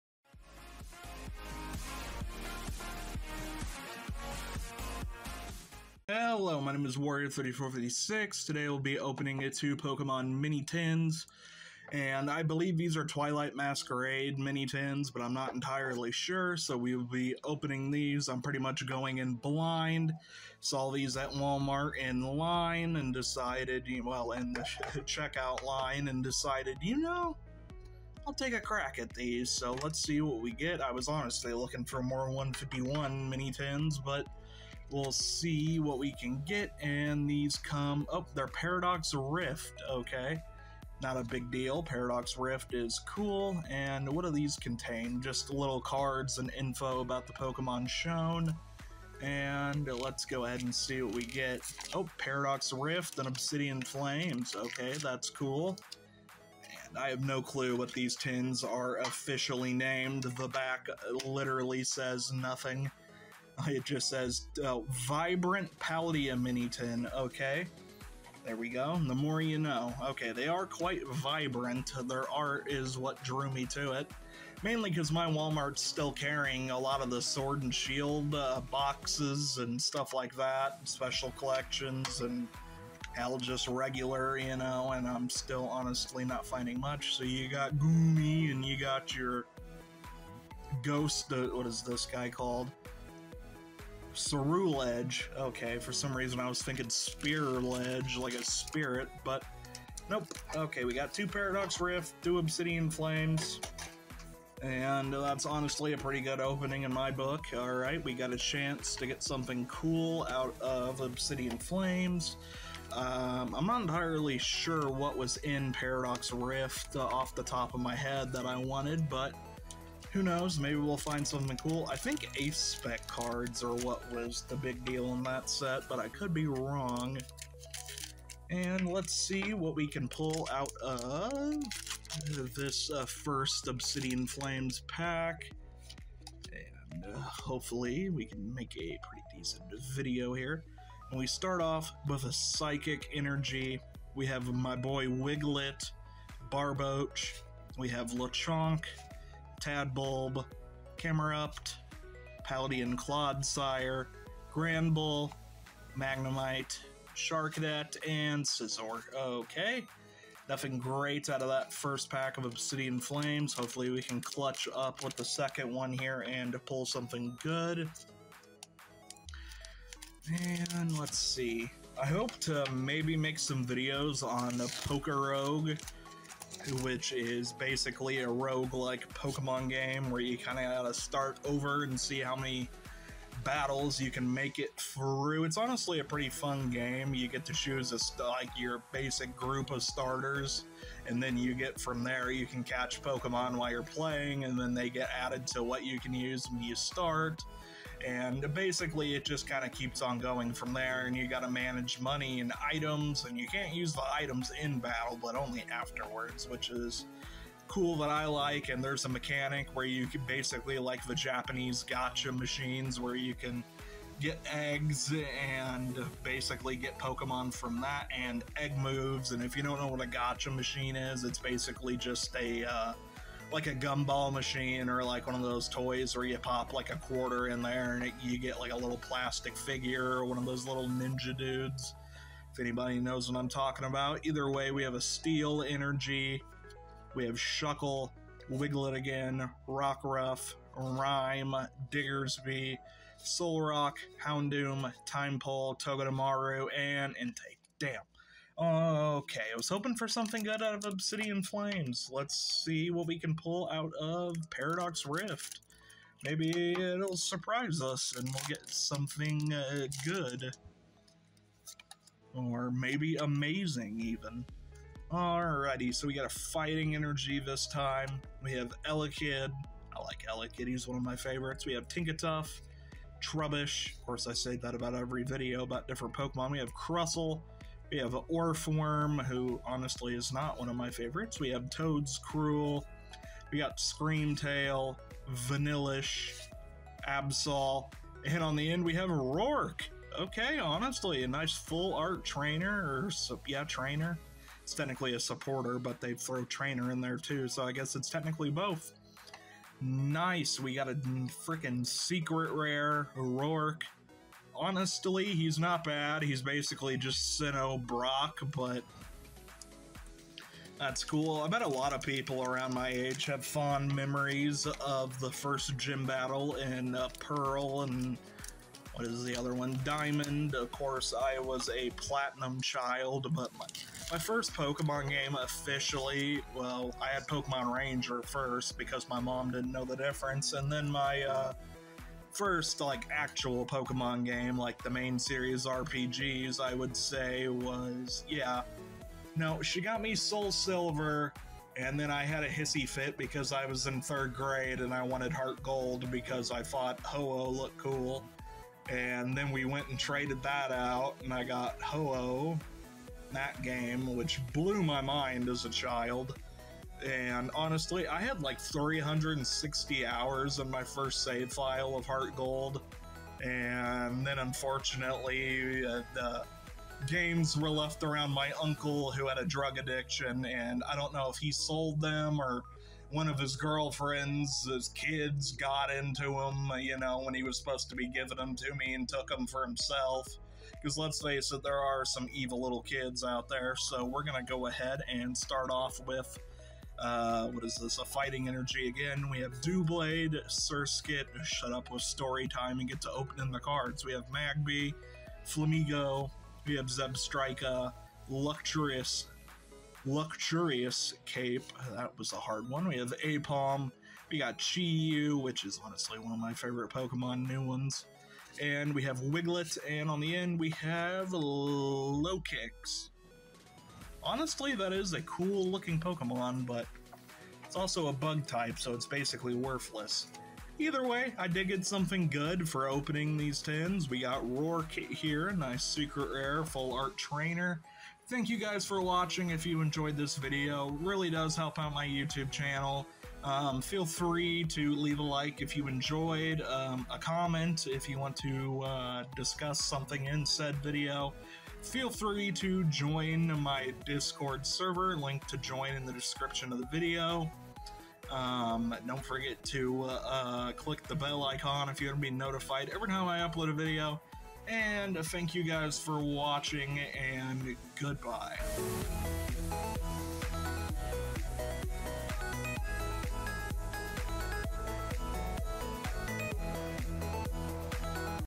Opening Two Pokemon Vibrant Paldea Mini Tins!
Hello today we open two pokemon vibrant paldea mini tins!